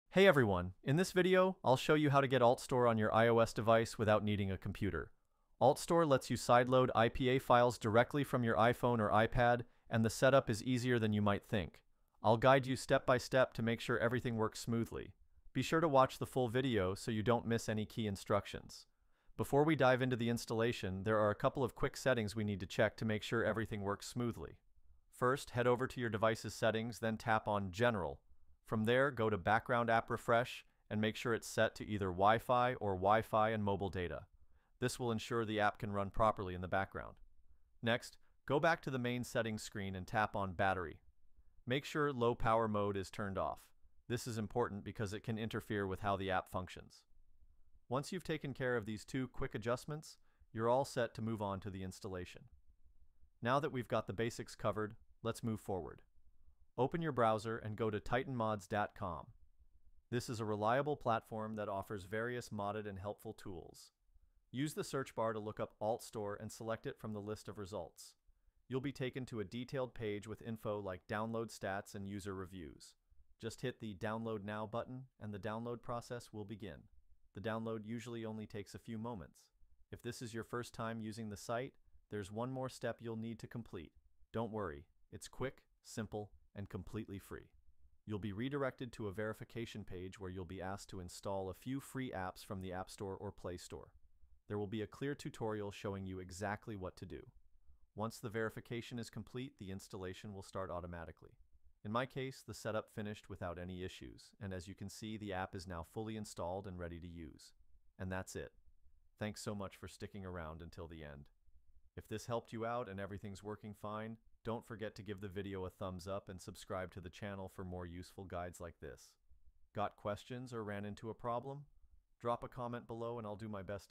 How to Get and Install Altstore ✅ Sideload IPAs to iOS (NO COMPUTER)
Want to sideload IPA files on your iPhone without using a computer?
In this video, I’ll show you exactly how to get and install AltStore directly on your iOS device — no PC or Mac required.

I’ll guide you step-by-step through the full process so it’s simple and safe, even if you’ve never done this before.

📱 Works on iOS 18 and below
🔐 No jailbreak needed
🕒 Quick setup in just a few minutes